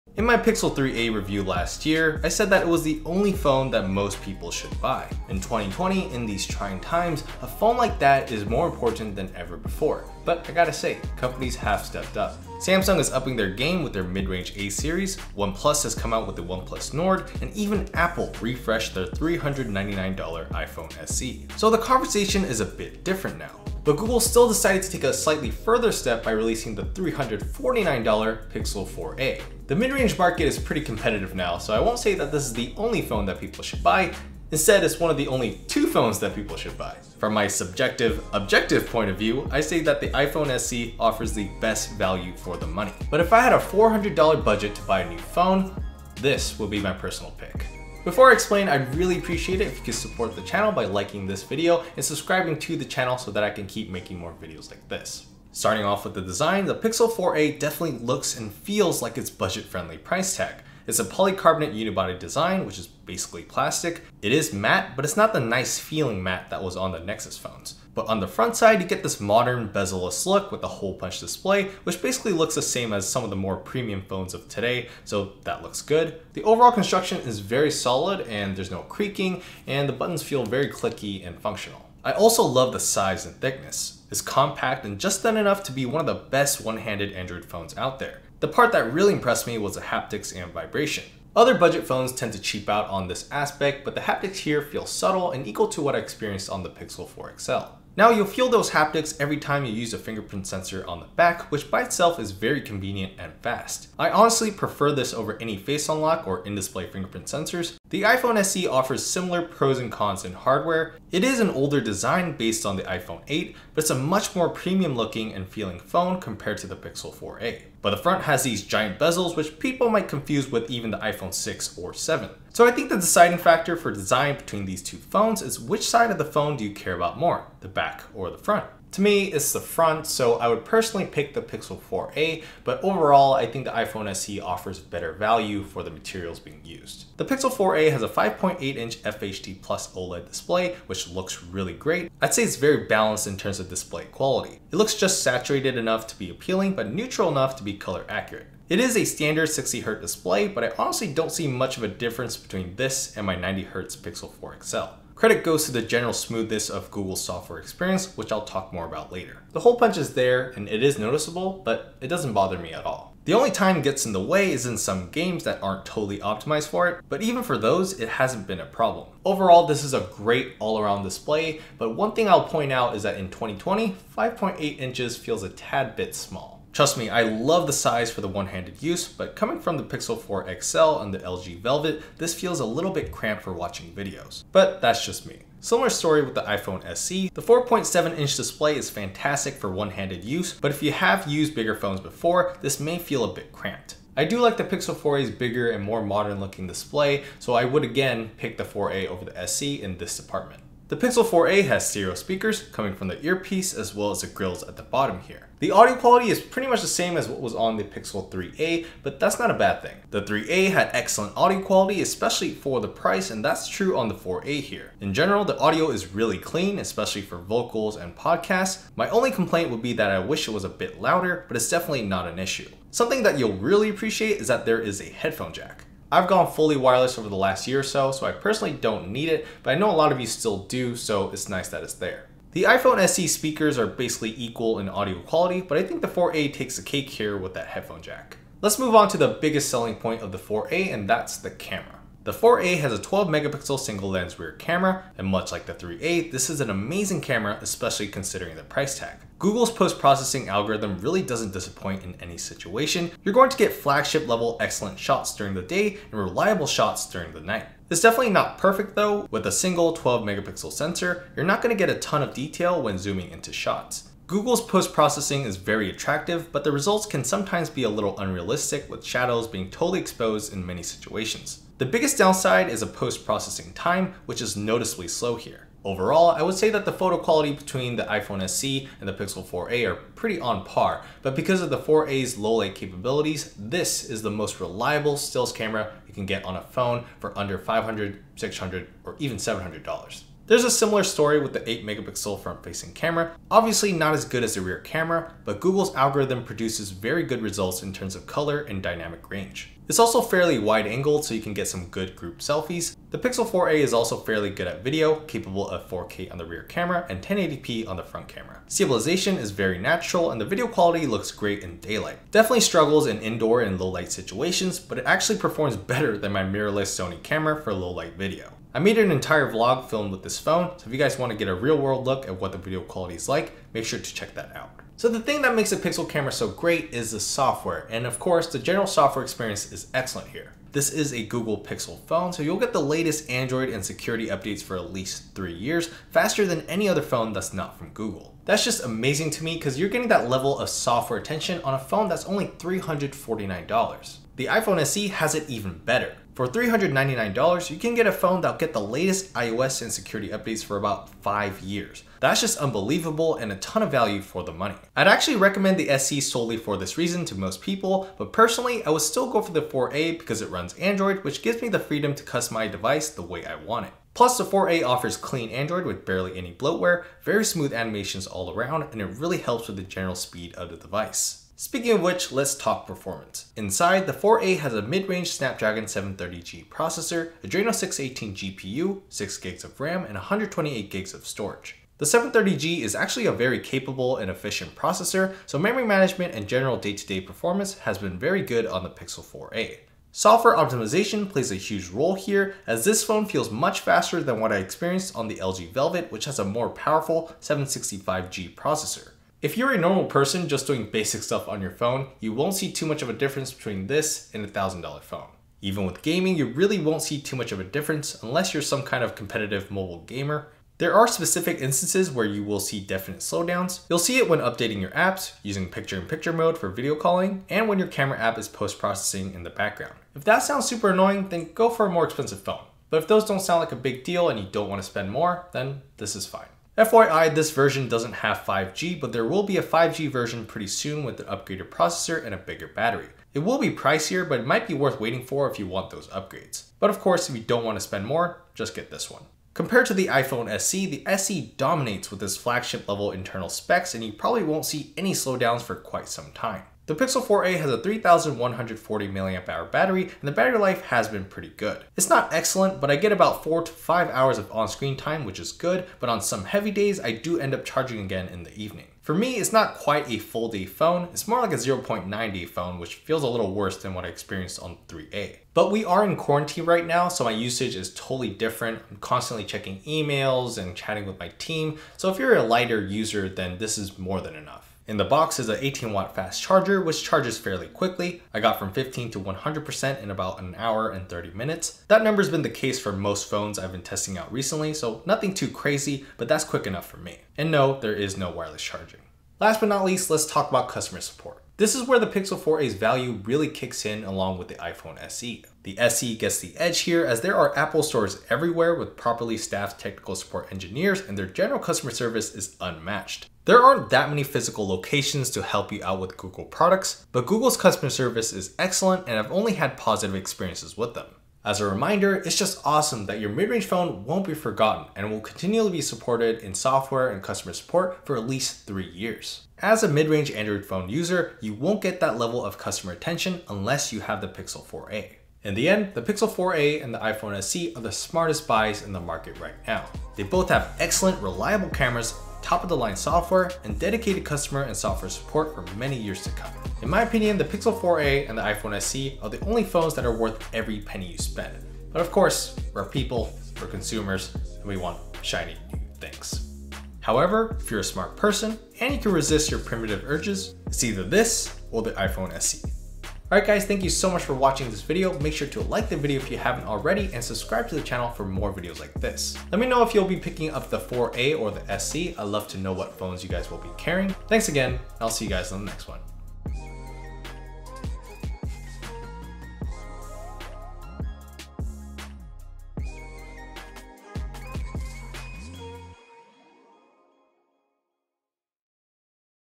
Pixel 4a vs. iPhone SE: Why I Picked the Pixel
The midrange smartphone market is ramping up with great offerings from Samsung, OnePlus, Apple, and Google. However, when it comes down to it, the only two phones that matter are the Google Pixel 4a and the Apple iPhone SE (2020). While I wholeheartedly admit the iPhone SE offers better overall value for the price, I would personally choose the Pixel 4a. Find out why in the video!

Google Pixel 4a (Paid Link)
Apple iPhone SE (2020) (Paid Link)